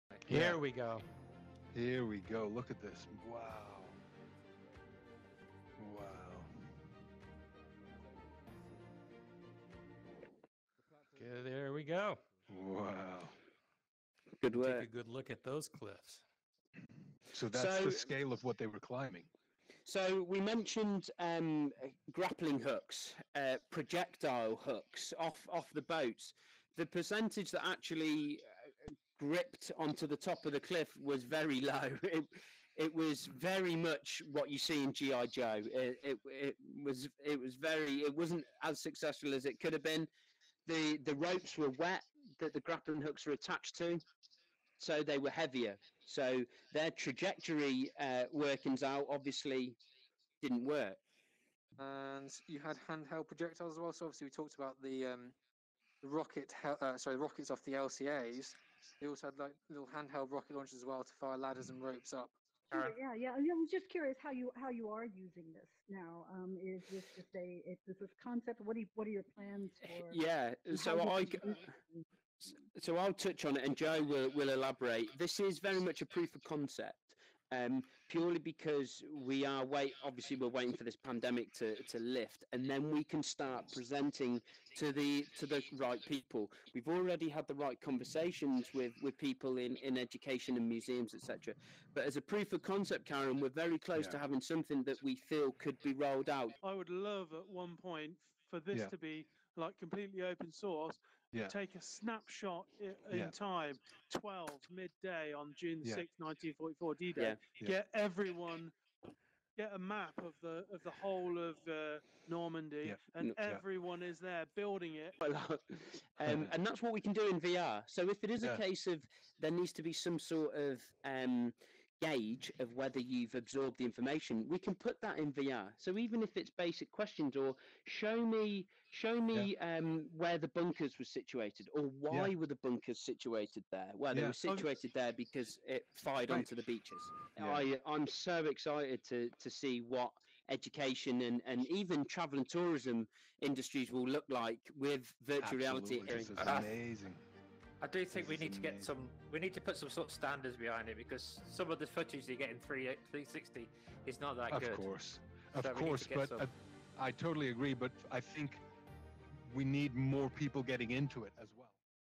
WW2 VR interactive history lesson on Pointe du hoc Normandy France
People recall information better through virtual reality
"Researchers found that people remember information better if it is presented to them in a virtual environment"

Loving the work of Immersive VR Education & Reality In Virtual Reality - was great to see early insights into concepts being developed for immersive learning environments, this example shared progress on WW2 VR interactive history lesson on Pointe du hoc Normandy France. Particularly liked the combination of 360 video/image and 3D models, and being able to interact with the environment, whilst listening to the historical information.

WW2 VR lesson on Pointe du hoc Normandy France: Live stream recording of the whole event here:

Thanks to the team who have been organising these sessions: Brian Waterfield Joe Harvey David W. Sime Chris Madsen Daniel Dyboski-Bryant Alex Foster Alex Harvey